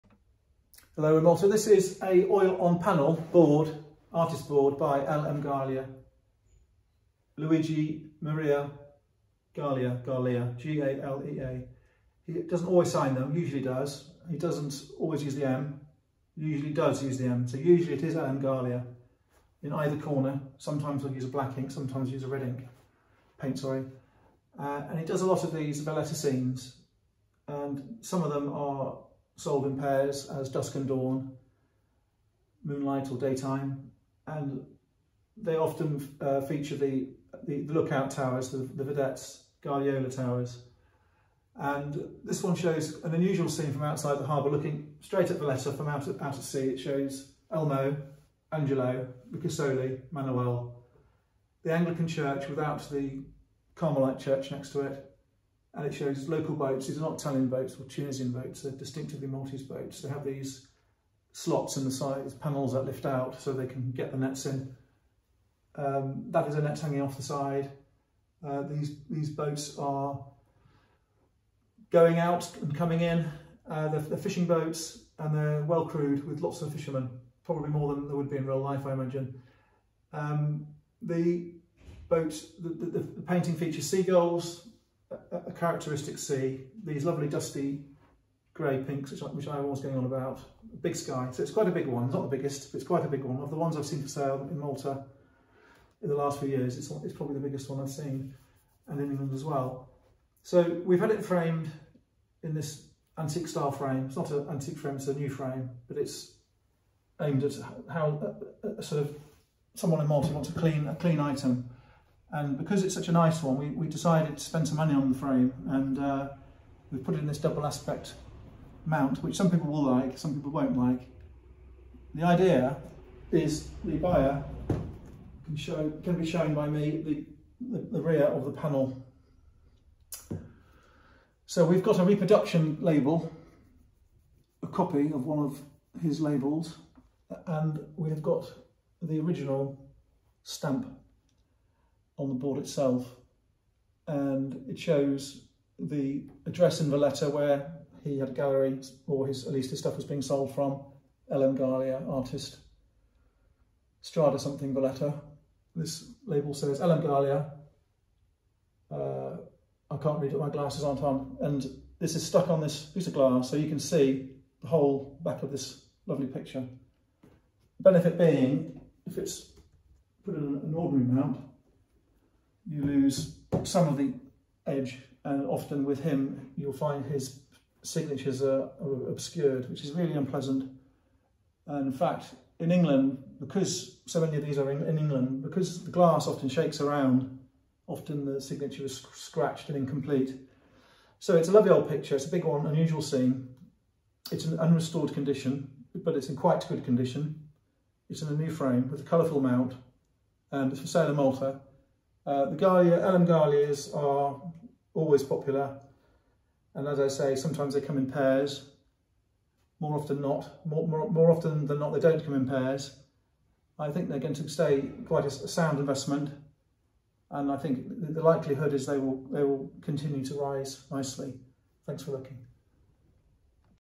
L.M. Galea ANTIQUE large oil on panel [ with Artist's backstamp ] fishing boats by Valletta Malta
General chat